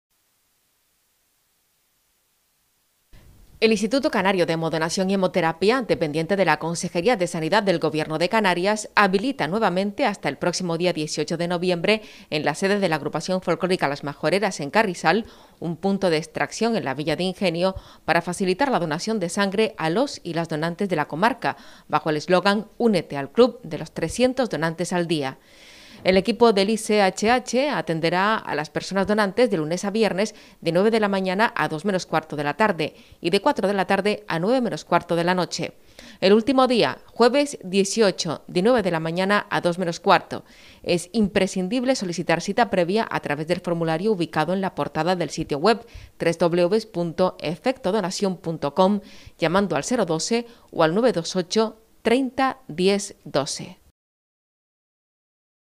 LA PLAZA DE LAS MAJORERAS VUELVE A ACOGER UN PUNTO DE DONACIÓN DE SANGRE HASTA EL DÍA 18 DE ESTE MES
LA PLAZA DE LAS MAJORERAS VUELVE A ACOGER UN PUNTO DE DONACIÓN DE SANGRE HASTA EL PRÓXIMO DÍA 18 DE ESTE MES

El Instituto Canario de Hemodonación y Hemoterapia (ICHH), dependiente de la Consejería de Sanidad del Gobierno de Canarias, habilita nuevamente hasta el próximo día 18 de noviembre en la sede de la Agrupación Folklórica Las Mahoreras, en Carrizal, un punto de extracción en la Villa de Ingenio para facilitar la donación de sangre a los y las donantes de la comarca bajo el eslogan ‘Únete al club de los 300 Donantes al día’.

El equipo del ICHH atenderá a las personas donantes de lunes a viernes de 9.00 a 13.45 y de 16.00 a 20.45 horas, el último día, jueves 18, de 9.00 a 13.45 horas. Es imprescindible solicitar cita previa a través del formulario ubicado en la portada del sitio web llamando al 012 o al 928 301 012.

Antes de acudir a donar, es recomendable revisar la información contenida en las pestañas de ‘Información’ y ‘Protocolo’ en la misma página web, para familiarizarse con las medidas de seguridad establecidas.

Cabe recordar que la Villa de Ingenio continúa siendo Sede Provincial del Día Mundial del Donante de Sangre por su compromiso durante los últimos años.